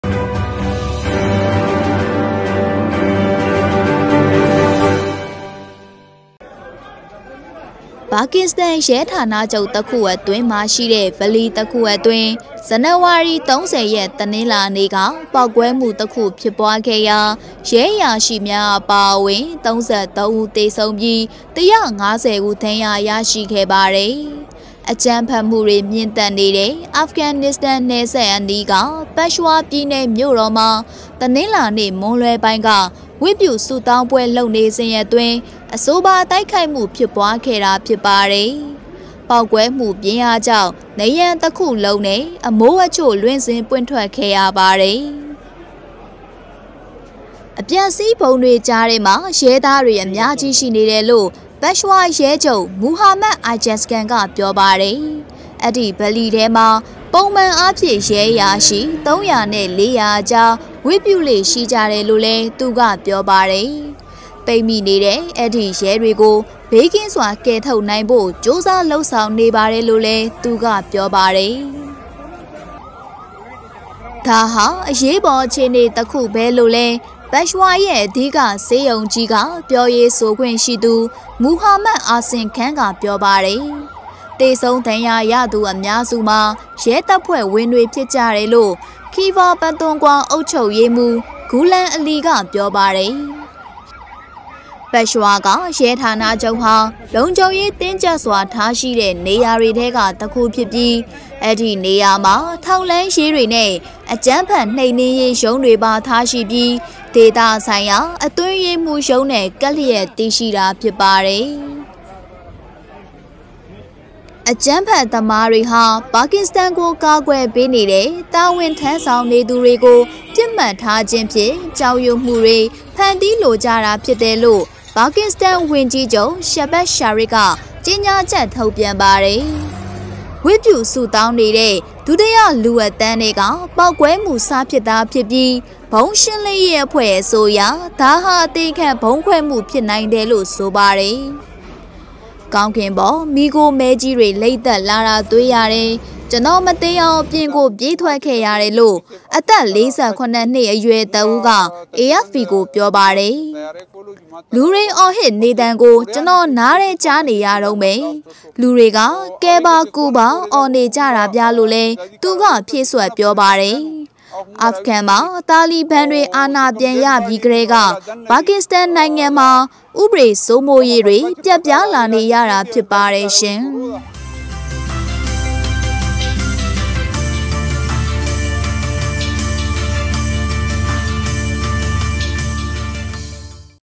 ပါကစ္စတန် ရဲဌာနချုပ်တစ်ခုအတွင်းရှိ ဗလီတစ်ခုတွင် အသေခံဗုံးခွဲမှုကြောင့် ၃၃ဦးသေဆုံး
ပါကစ္စတန်ရဲဌာနချုပ်တစ်ခုအတွင်းမှာရှိတဲ့ ဗလီတစ်ခုအတွင်း  ဇန်နဝါရီ ၃၀ ရက် တနင်္လာနေ့က ပေါက်ကွဲမှုတစ်ခုဖြစ်ပွားခဲ့ရာ ရဲအရာရှိများအပါအဝင် ၃၃ ဦးသေဆုံးပြီး ၁၅၀ ဦးဒဏ်ရာရရှိခဲ့ပါတယ်။
အကြမ်းဖက်မှုတွေမြင့်တက်နေတဲ့ အာဖဂန်နစ္စတန်နယ်စပ်အနီးက  ပက်ရှ်ဝါပြည်နယ်မြို့တော်မှာ တနင်္လာနေ့ မွန်းလွဲပိုင်းက ဝတ်ပြုဆုတောင်းပွဲလုပ်နေစဉ်အတွင်း အဆိုပါတိုက်ခိုက်မှုဖြစ်ပွားခဲ့တာဖြစ်ပါတယ်။
ပေါက်ကွဲမှုပြင်းအား ကြောင့် နံရံတစ်ခုလုံးနှင့် အမိုးအချို့ လွင့်စင်ပွင့်ထွက်ခဲ့ရပါတယ်။
Source AFP